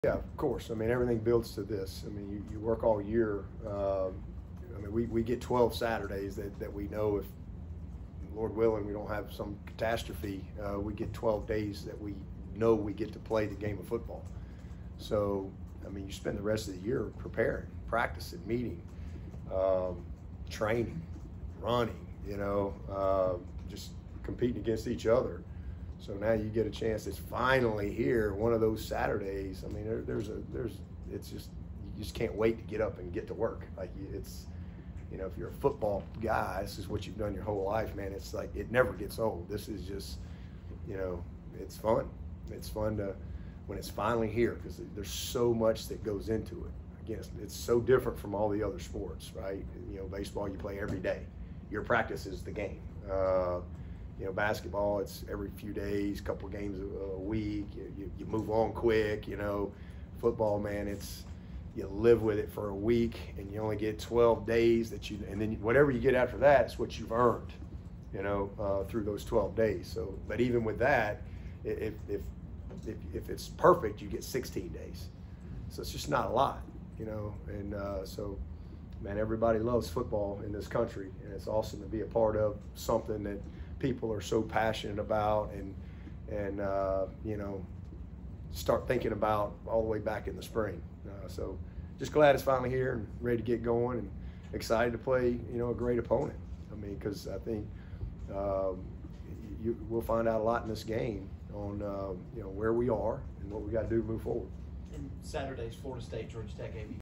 Dabo Swinney "Everything builds to this"
Clemson head coach Dabo Swinney explains the importance and excitement of the 2024 season opener.